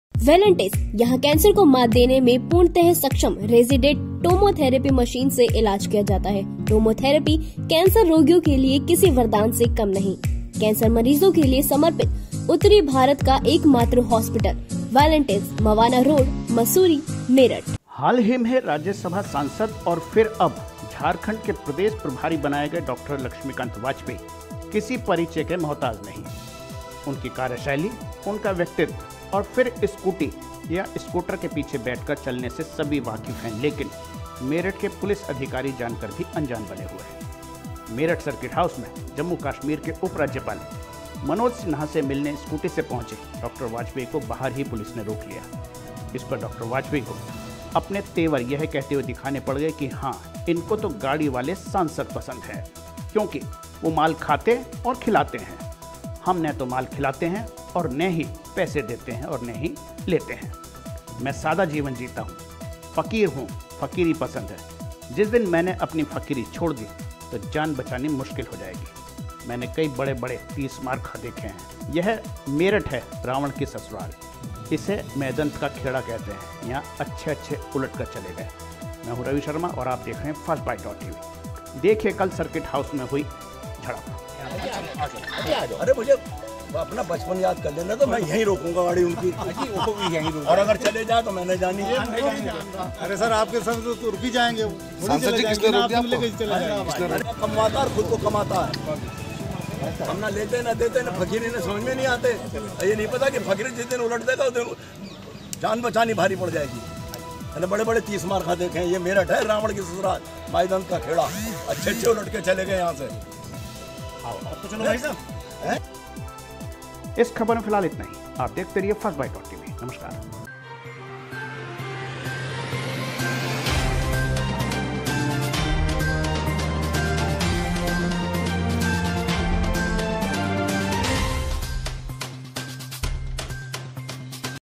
तमतमाये डा.लक्ष्मीकांत बोले : ट्रांसफ़र नहीं कराता, यहीं मूंग दलूंगा छाती पर || First byte tv
रोकने पर जमकर पुलिस पर जमकर बरसे लक्ष्मीकांत
उपराज्यपाल से मिलने पहुँचे थे सर्किट हाउस
पुलिस क्षेत्राधिकारी अरविंद चौरसिया ने हाथ जोड़ते हुए की मनुहार
बोले पुलिस को पैसे देने व लेने वाले सांसद पसंद हैं
न ही तो मैं पैसे लेता हूँ और न ही देता हूँ : लक्ष्मीकान्त
किस सांसद पर तंज कस गये डा.वाजपेयी